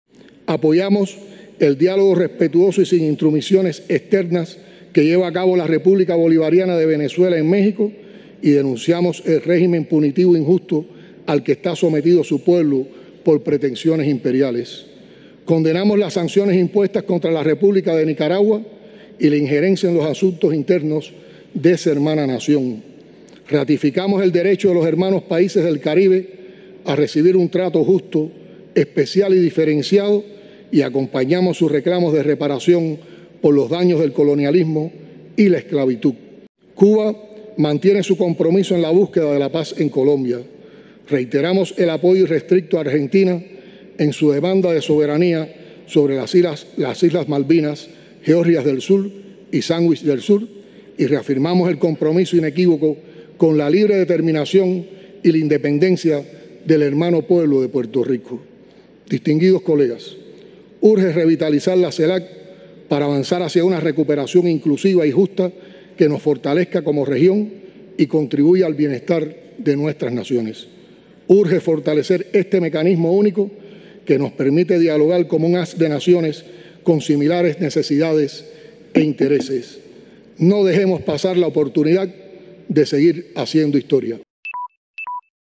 En la CumbreDeDictadores o CELAC, Díaz-Canel 🇨🇺 apoya al Gobierno de Venezuela, Nicaragua 🙄
Durante la CumbreDeDictadores o CELAC, el dictador Díaz-Canel 🇨🇺 apoya al Gobierno de Venezuela, Nicaragua y Argentina y pide que la paz en Colombia sea una realidad.